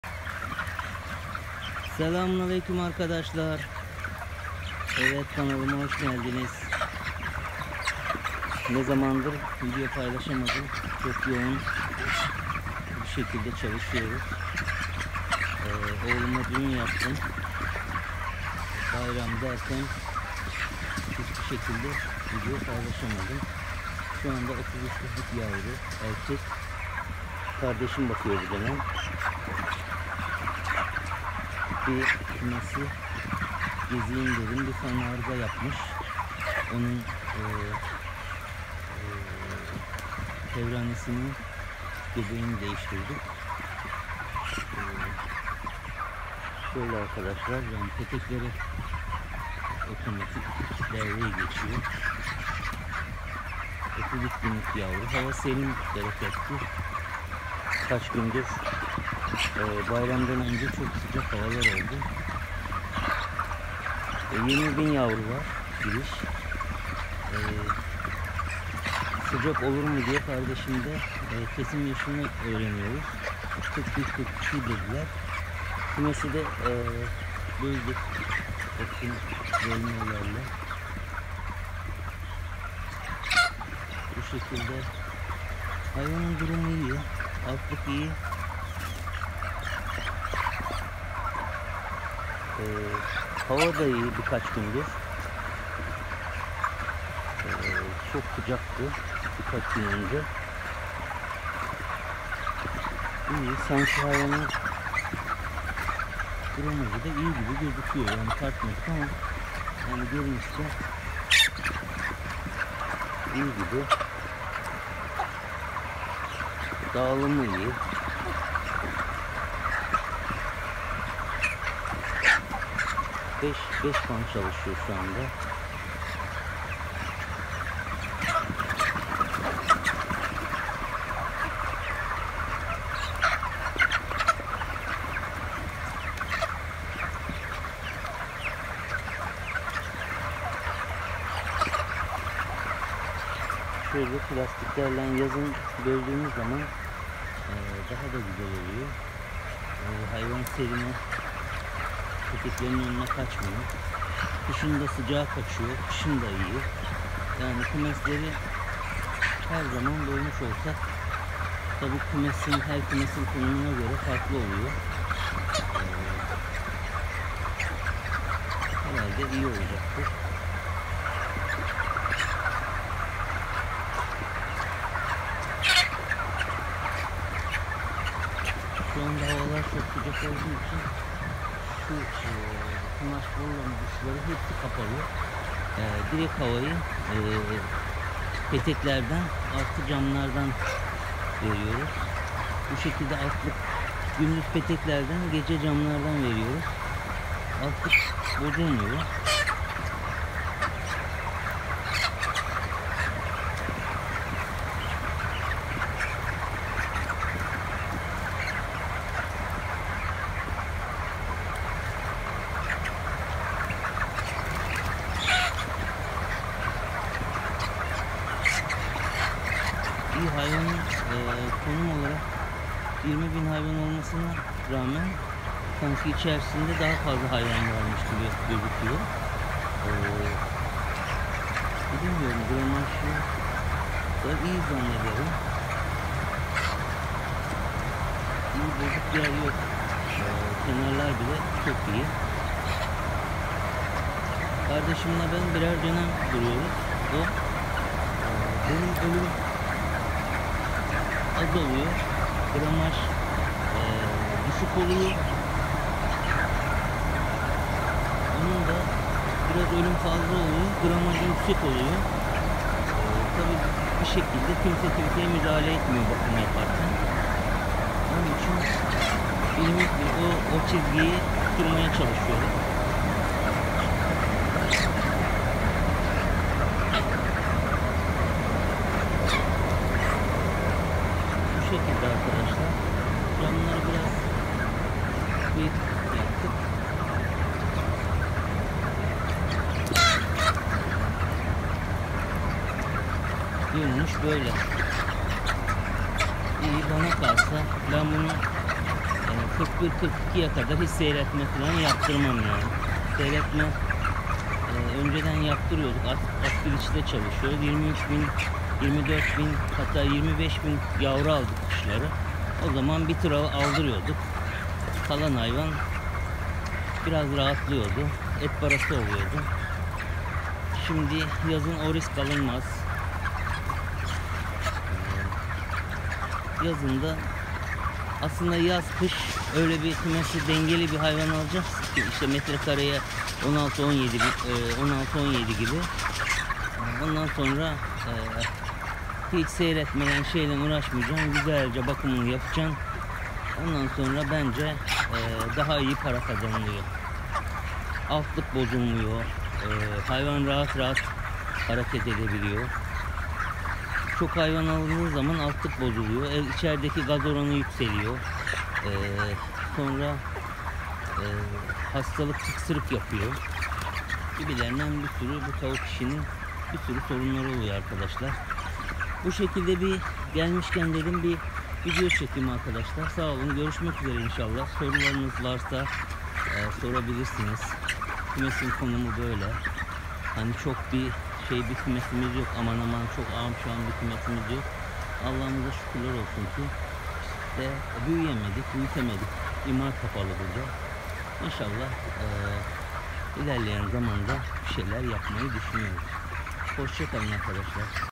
TAVUK ÇİFTLİĞİNDEN GÖRÜNTÜLER.
Arkadaşlar bu videomuzda 33 günlük erkek piliçlerimizin genel durumunu cektik iyi seyirler.